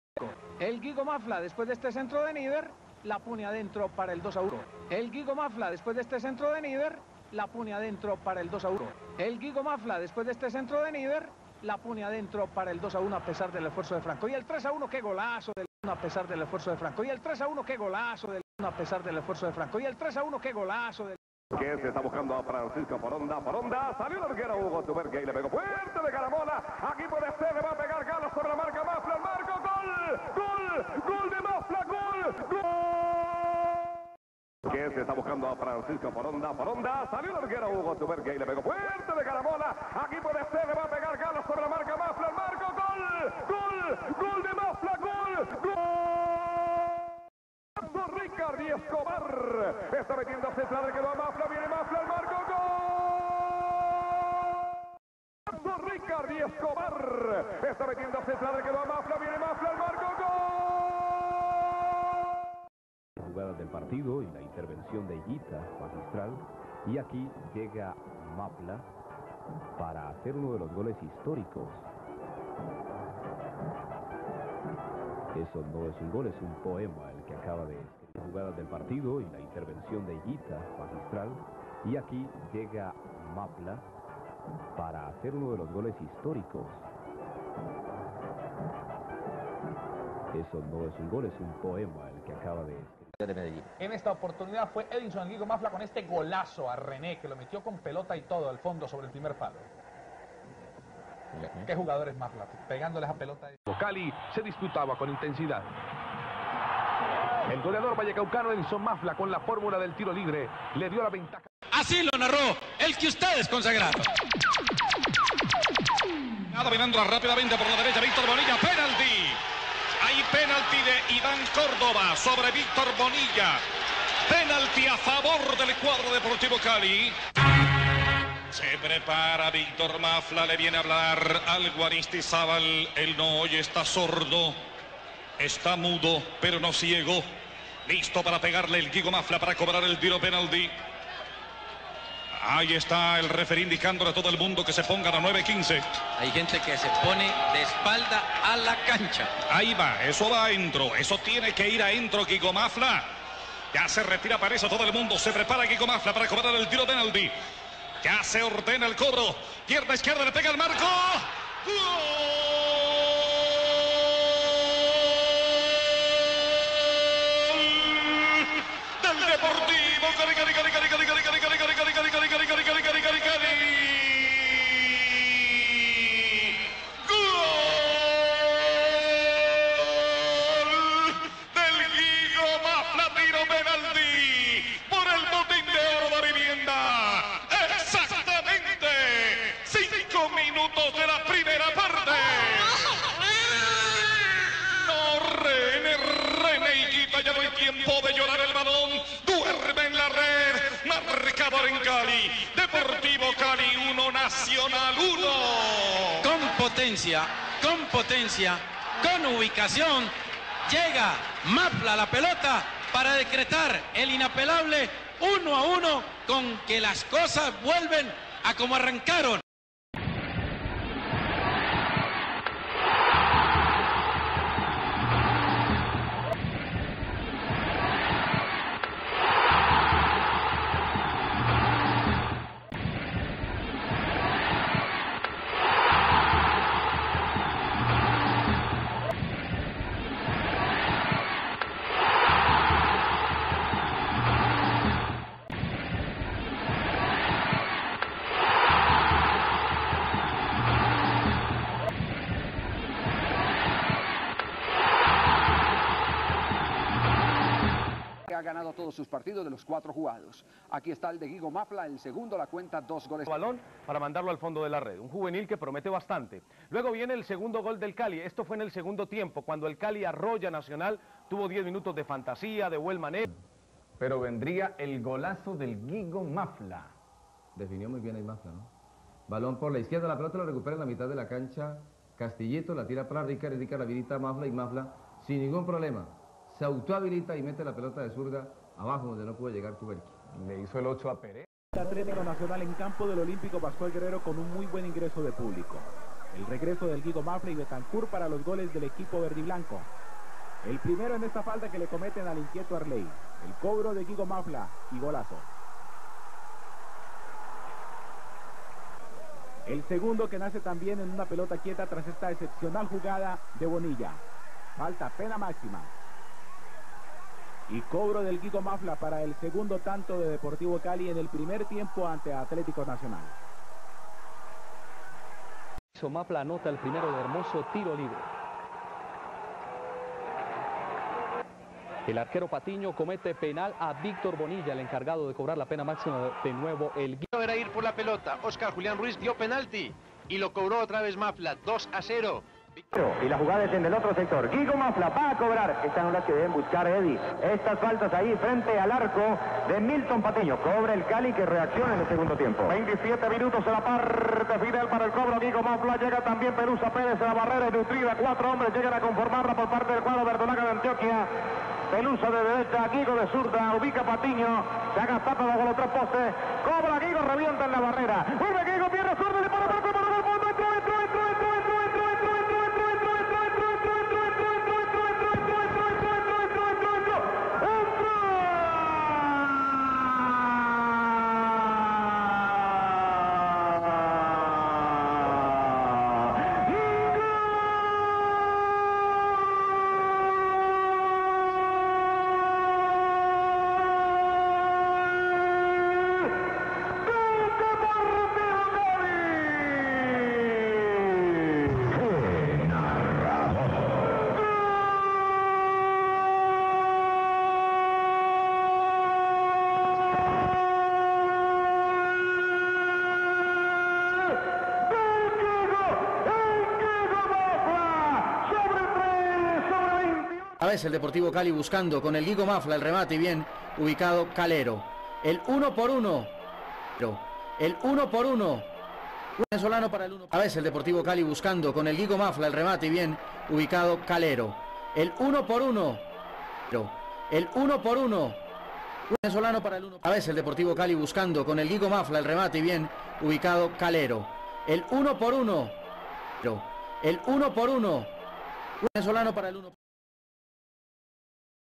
GOLES DE EDISON MAFLA DEL DEPORTIVO CALI AL ATLÈTICO NACIONAL 1993-1999
El goleador frente al Atlètico Nacional en Deportivo Cali es Edison Guigo Mafla que cuenta con 16 tantos.